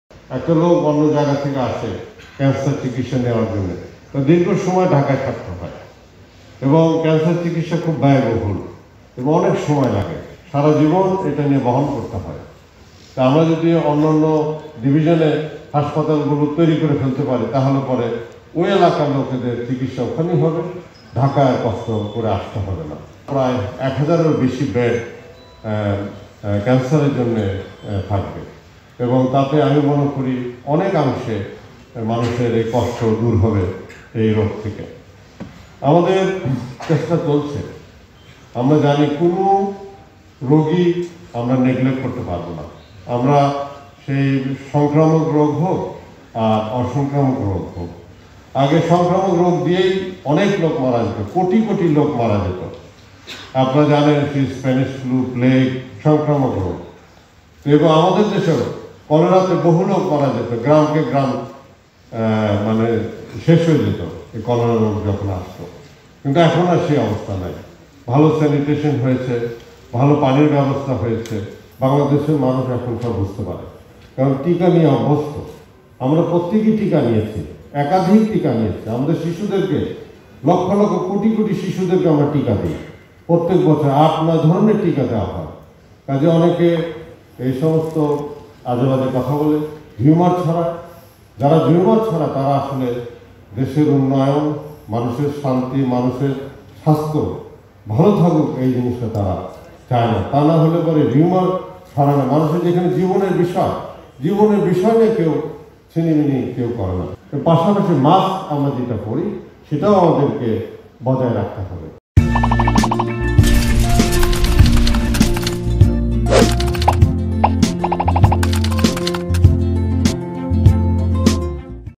৮ বিভাগেই ক্যানসার হাসপাতাল হচ্ছে : স্বাস্থ্যমন্ত্রী | Jagonews24.com
৮ বিভাগেই ক্যানসার হাসপাতাল হচ্ছে : স্বাস্থ্যমন্ত্রী